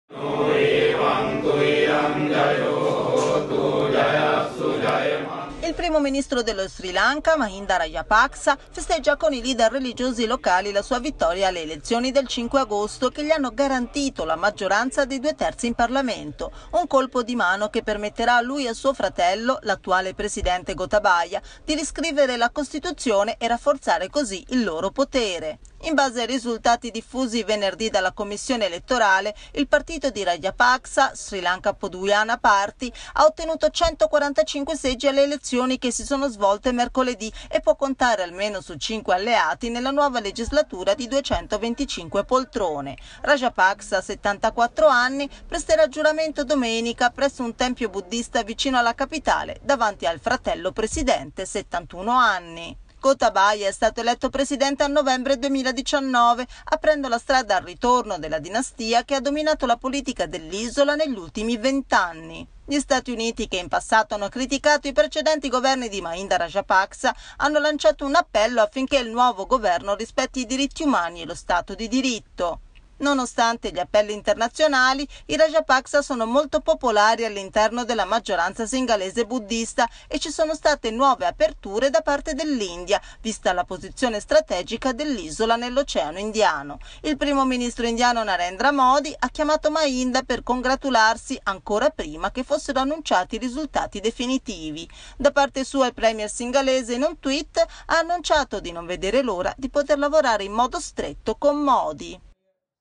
In Sri Lanka i due fratelli Rajapaksa sempre più potenti
Roma, 7 ago. (askanews) - Il primo ministro dello Sri Lanka Mahinda Rajapaksa festeggia con i leader religiosi locali la sua vittoria alle elezioni del 5 agosto, che gli hanno garantito la maggioranza dei due terzi in Parlamento. Un colpo di mano che permetterà a lui e a suo fratello, l'attuale presidente Gotabaya, di riscrivere la costituzione e rafforzare così il loro potere.
In base ai risultati diffusi venerdì dalla Commissione elettorale, il partito di Rajapaksa, Sri Lanka Podujana Party (SLPP), ha ottenuto 145 seggi alle elezioni che si sono svolte mercoledì e può contare almeno su 5 alleati nella nuova legislatura di 225 poltrone. Rajapaksa, 74 anni, presterà giuramento domenica presso un tempio buddista vicino alla capitale davanti al fratello, il 71enne Gotabaya, attuale presidente.
Gotabaya è stato eletto presidente a novembre 2019, aprendo la strada al ritorno della dinastia che ha dominato la politica dell'isola negli ultimi 20 anni.
Gli Stati Uniti, che in passato hanno criticato i precedenti governi di Mahinda Rajapaksa (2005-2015), hanno lanciato un appello affinché il nuovo governo rispetti i diritti umani e lo stato di diritto.
Nonostante gli appelli internazionali, i Rajapaksa sono molto popolari all'interno della maggioranza singalese-buddista e ci sono state nuove aperture da parte dell'India, vista la posizione strategica dell'isola nell'Oceano indiano. Il primo ministro indiano Narendra Modi ha chiamato Mahinda per congratularsi ancora prima che fossero annunciati i risultati definitivi. Da parte sua il premier singalese in un tweet ha annunciato di non vedere l'ora di potere lavorare in modo stretto con Modi.